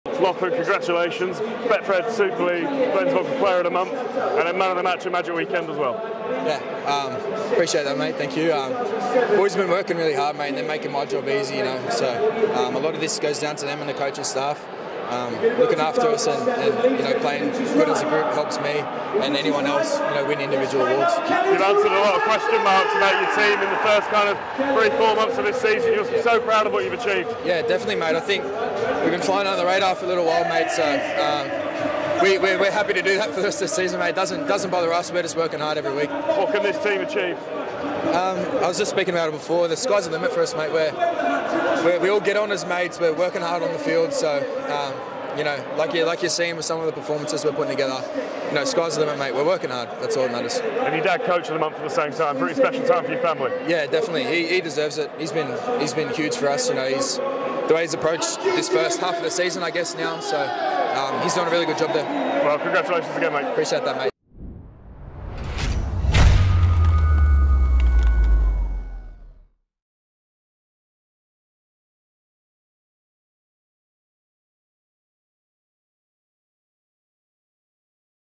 MagicWeekend - Lachlan Lam | Glens Player of The Month | Leigh Leopards.
It's been a pretty good day for the Lams. A win a Magic weekend, Adrian named Betfred Coach of the Month, and Lachlan named Glenns Player of The Month.

We caught up with the Leopards Halfback pitchside at St James Park/

The Sportsman